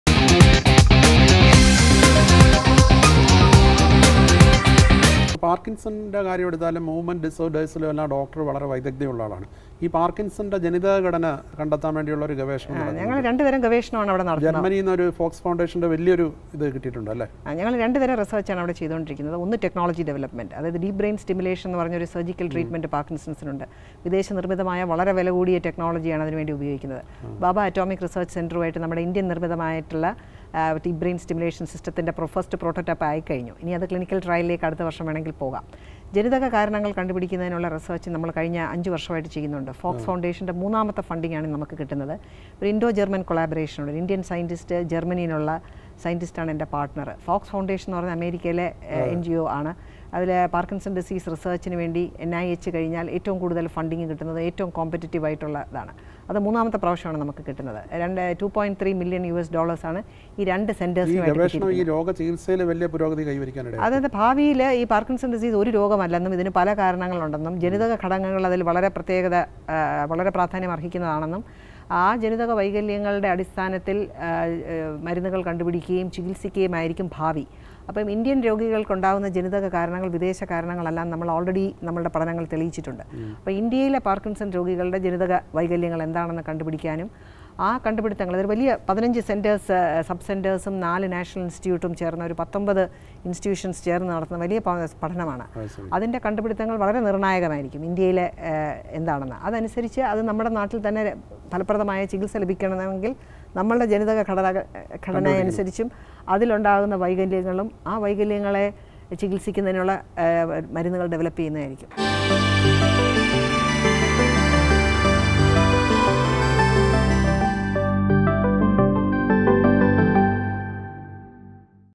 Diagnosis and treatment of parkinson's disease | Dr Asha Kishore | Straight Line
Asha Kishore, Professor of Neurology,Director of the Sree Chitra Tirunal Institute for Medical Sciences and Technology (SCTIMST).Dr. Kishore is the first woman to head the SCTIMST.